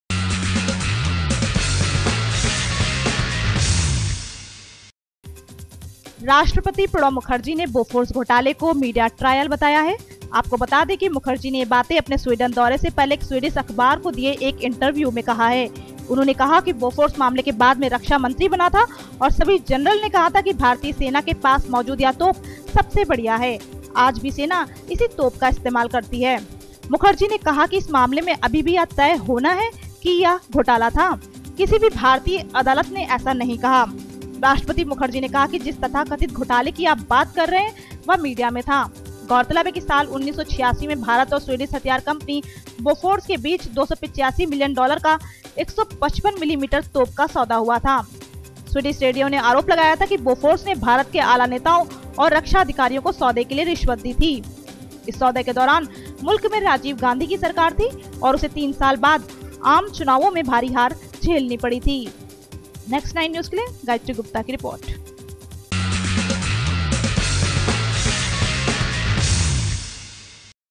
देखिये क्या था 1986 का बोफोर्स स्कैंडल Bofors Scandal was a Media Trial
Bofors Scandal was a Media Trial, No Evidence: Pranab Mukherjee Reveals in Swedish Paper
In 1986 there was a deal of $ 285 million, 155 mm cannon between India and Swedish arms firm, Bofors. Swedish Radio alleged that the Bofors deal was not pure where Indian politicians and defense officers took bribe for that. Now President Pranab Mukherjee reveals in Swedish paper that Bofors scandal was just a media trial there is no evidence.